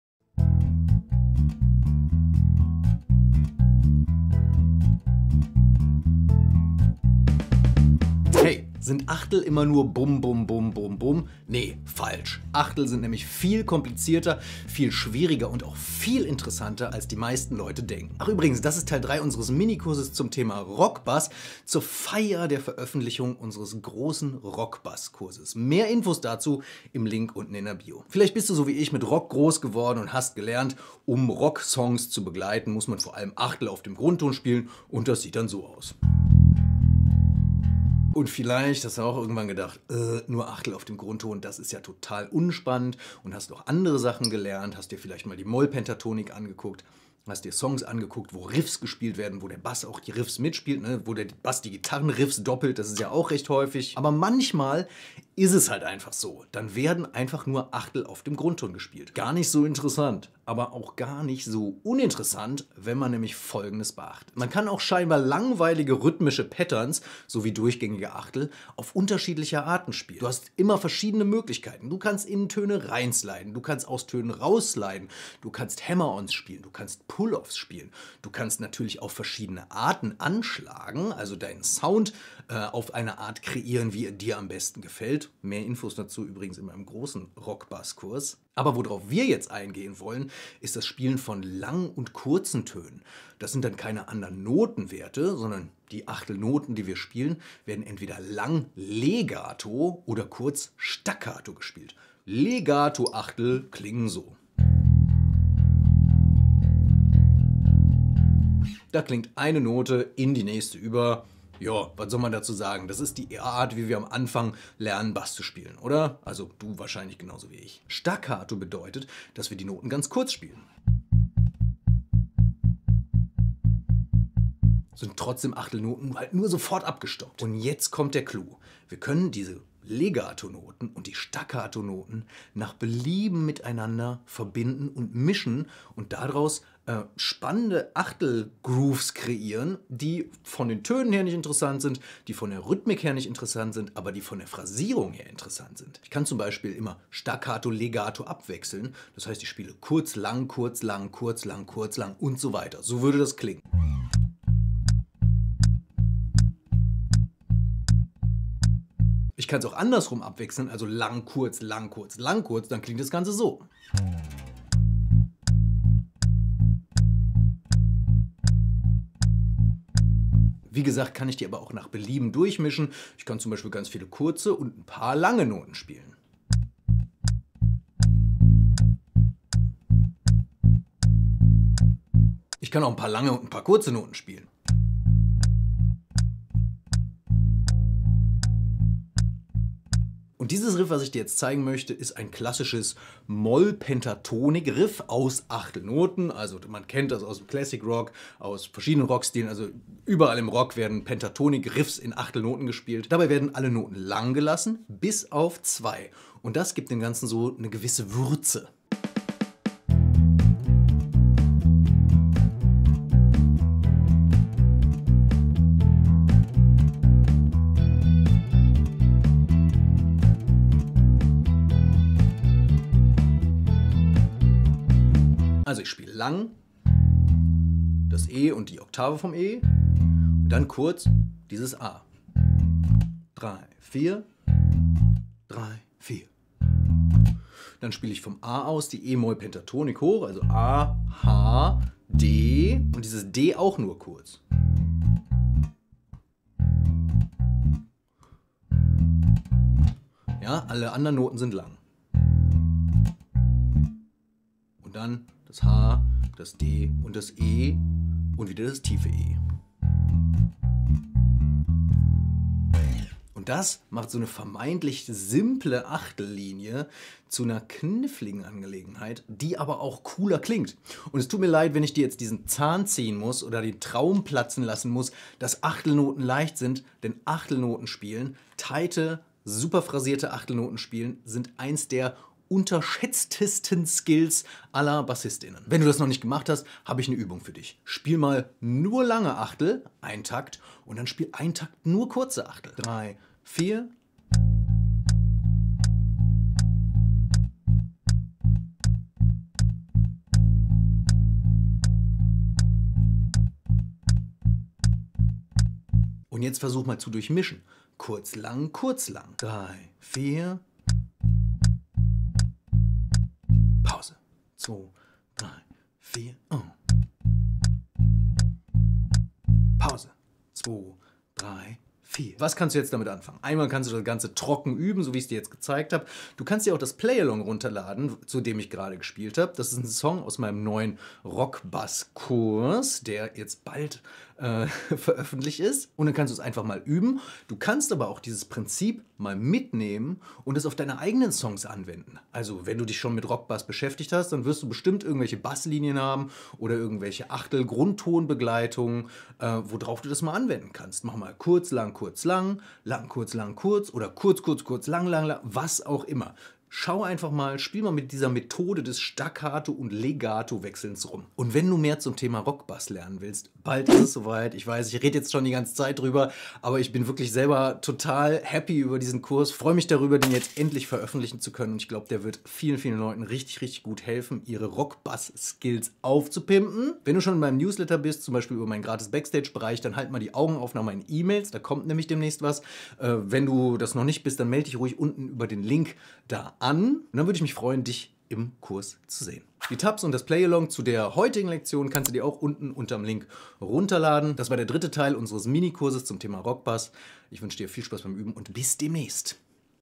Nie mehr langweilige Achtelnoten im Rock - E-Bass lernen
Achtung, der neue Rockbass Online Kurs ist jetzt eröffnet!

In dieser Rockbass Lektion zeige ich dir einen simplen Trick, mit dem du langweilige Achtenoten in spannende Bass Grooves verwandeln kannst.

Damit sind die Rock Lektionen aber noch längst nicht vorbei. Nächste Woche bringe ich dir drei coole Rock Songs bei, in denen du das Gelernte aus den letzten Videos praktisch anwenden kannst.